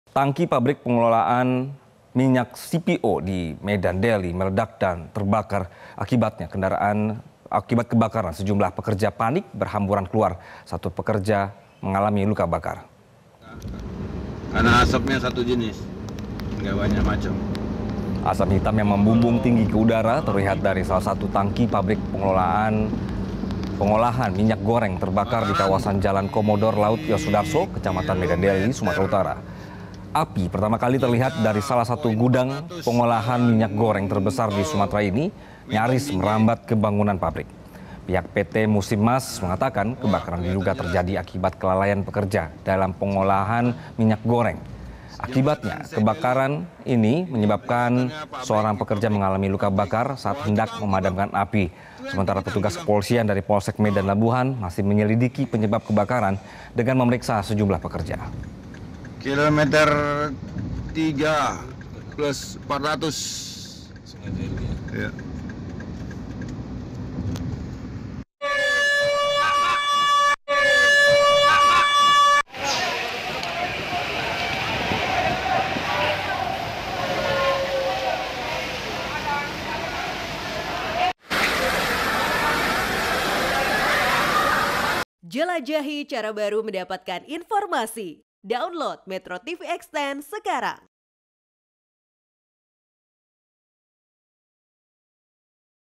Tangki Pengolahan Minyak Goreng di Medan Terbakar
MetroTV, Sumut, Belawan. Diduga lalai dalam bekerja, senin siang, tangki pabrik pengolahan minyak CPO di Medan Deli, meledak dan terbakar, akibat kejadian sempat membuat para pekerja panik berhamburan keluar.